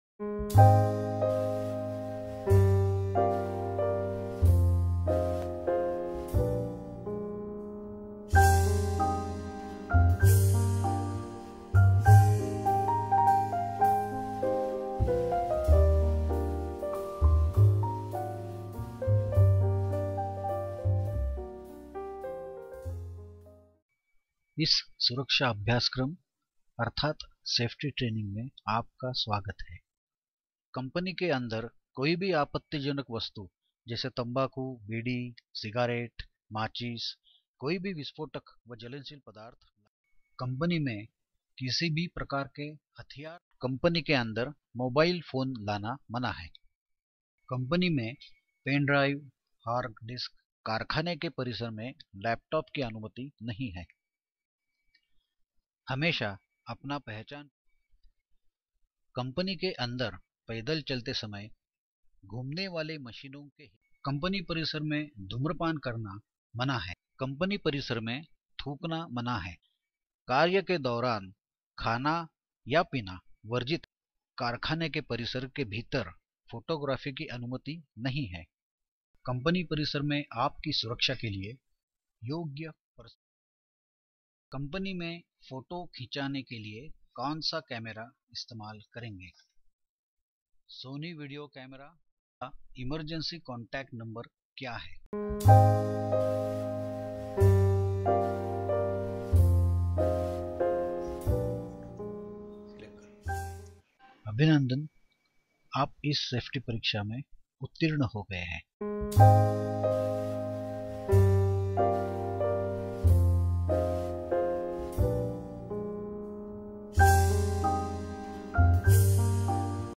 STK - Safety Training Kiosk
STK is a Safety Training KIOSK. It helps in creating a self-help training program with ease. STK comprises of two components i.e. a touch screen based KIOSK which can be operated by the user through a very user-friendly interface and a software which helps in creating and organising this program without any prior knowledge or training. STK is very useful for industries like chemicals, petrochemicals, pharma, IT. It’s an integrated system and hence comes equipped with all the necessary hardware / software and training modules.